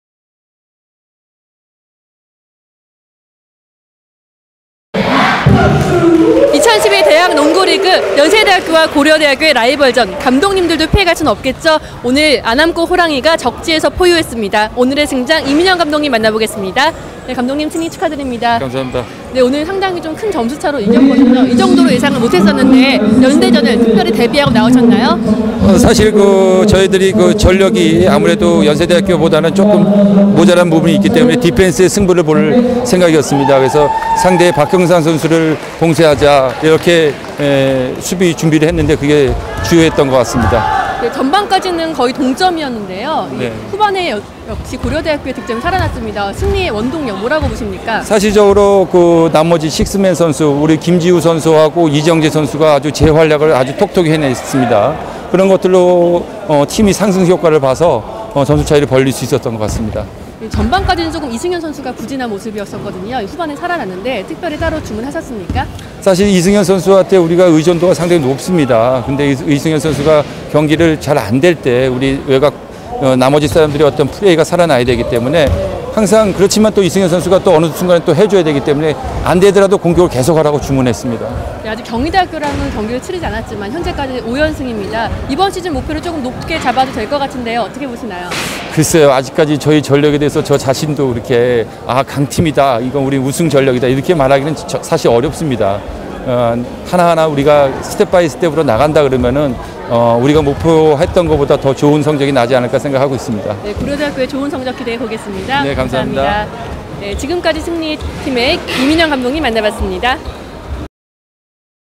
[대학리그] '라이벌전 승리' 고려대 이민형 감독 '식스맨들이 제활약 해줘'
안암골 호랑이의 포효가 신촌 하늘을 완전히 뒤덮었다.

고려대는 9일 연세대 신촌캠퍼스 체육관에서 열린 2012 KB국민은행 대학농구리그에서 3점슛 5개 포함 21점을 폭발시킨 김지후를 앞세워 '연적' 연세대를 79-66으로 꺾고 개막후 5연승을 내달렸다.

이날 승장 이민형 감독에게 연세대전을 앞두고 대비한 부분, 식스맨들이 제활약을 해줘 만족스런 모습을 보였다. 더불어 최근 좋은 분위기를 유지한 채 목표치 상승에 대해 언급하고 있다.